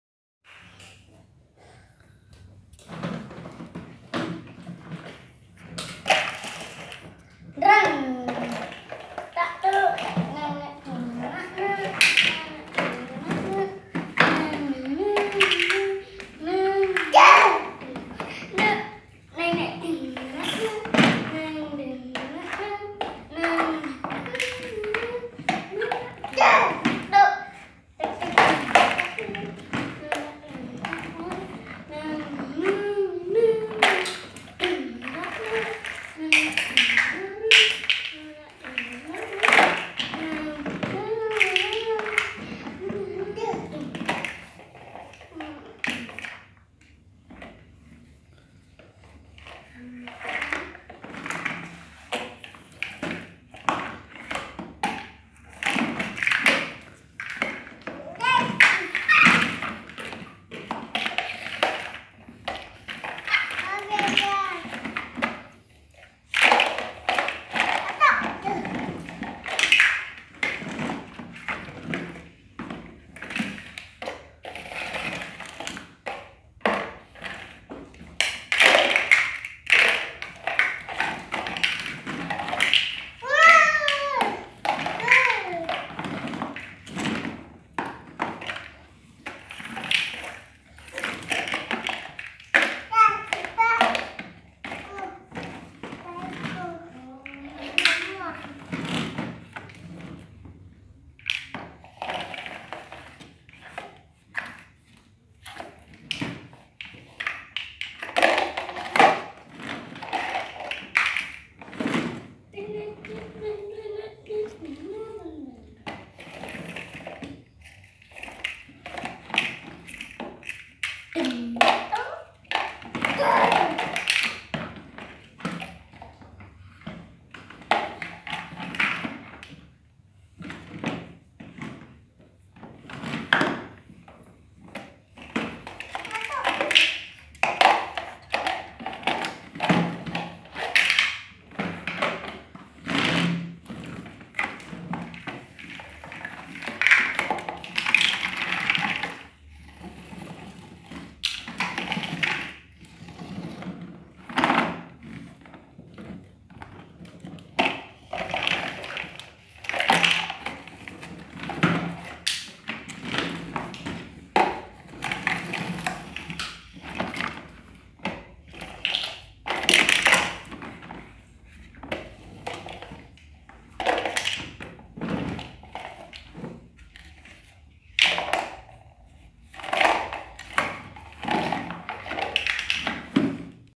It's So Funny How Kids Clean their house with excavator
It's So Funny How Kids Clean their how with excavator